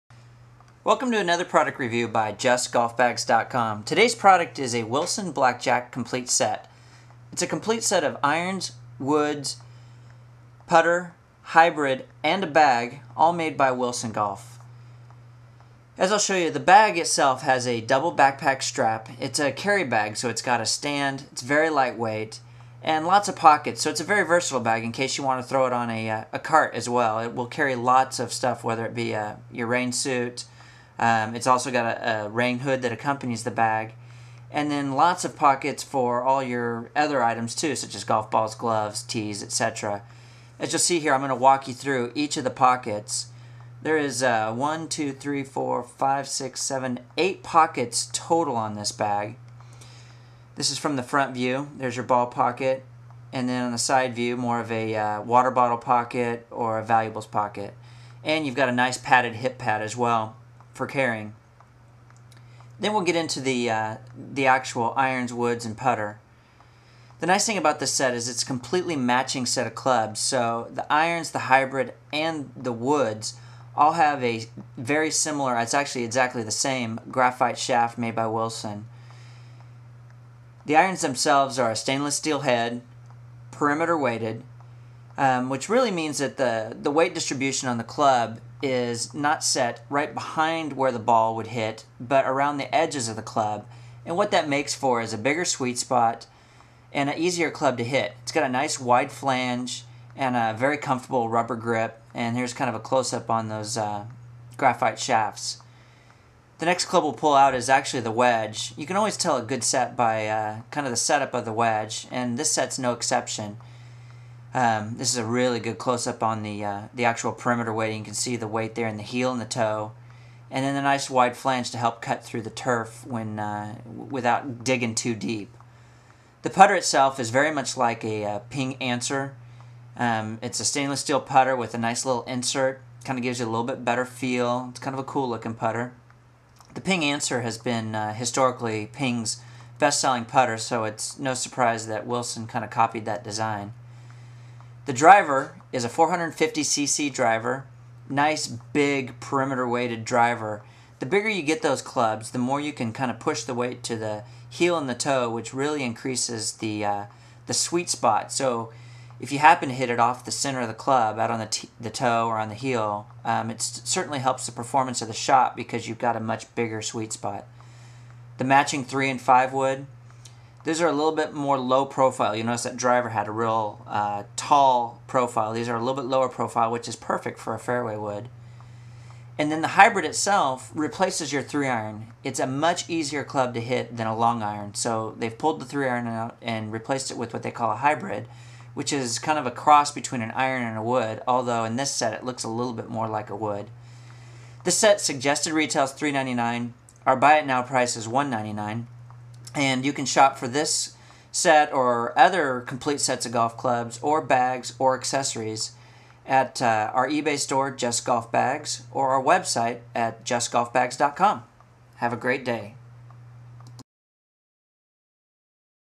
Wilson Black Jack Golf Club Review
A review of the Black Jack Complete Golf Club set by JustGolfBags.com